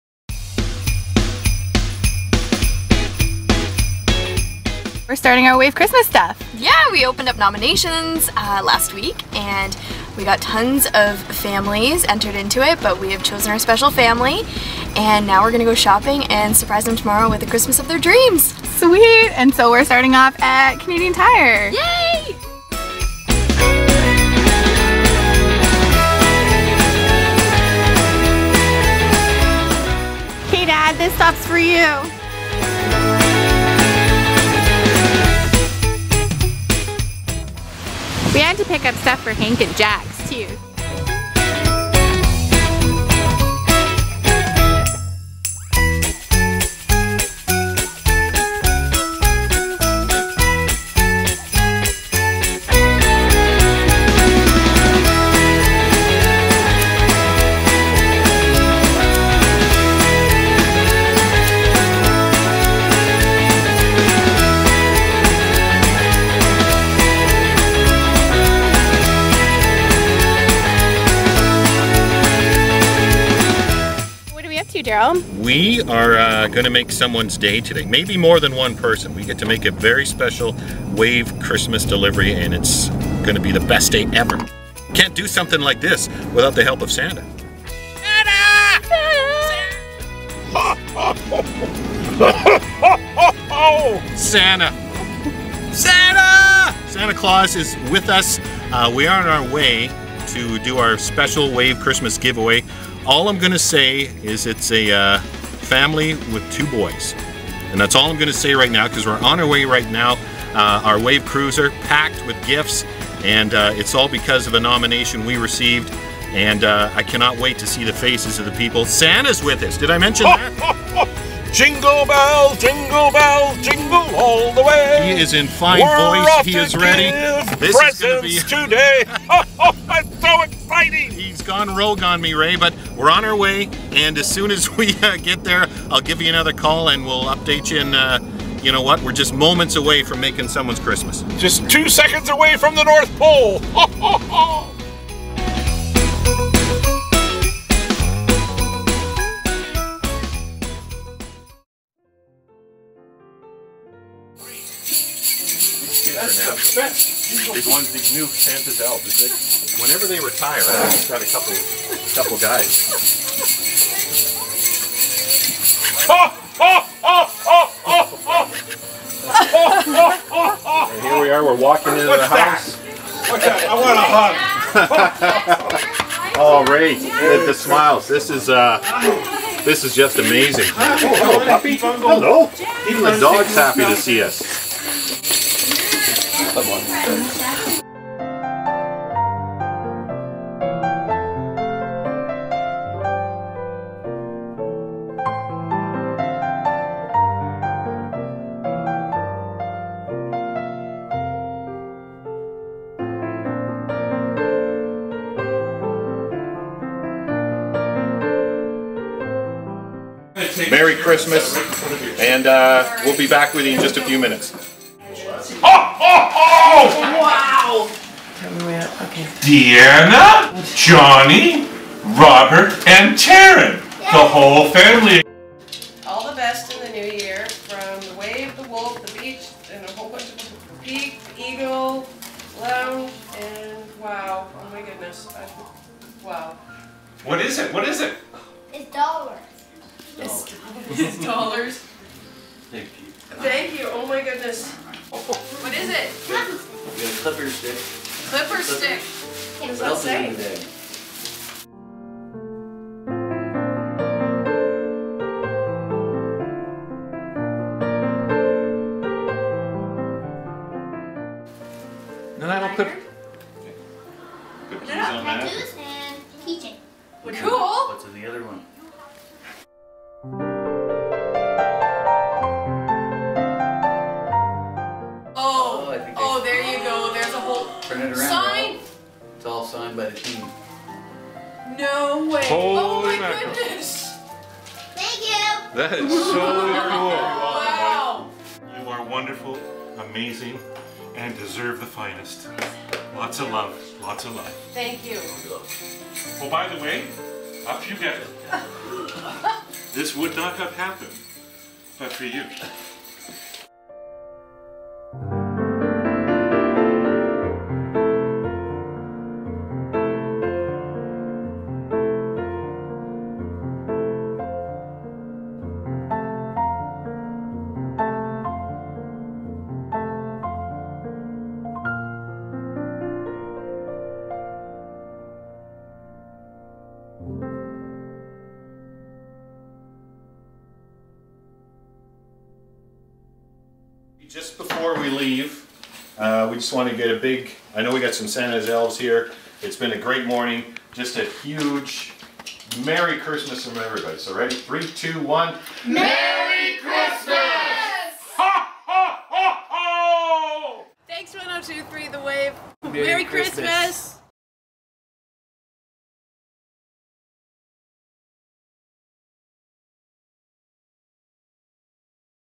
The Wave's Christmas Surprise
We teamed up with the Nanaimo Toy Run, Quality Foods, and with a little help from Avalon Cinemas, we brought one Nanaimo family some Christmas cheer!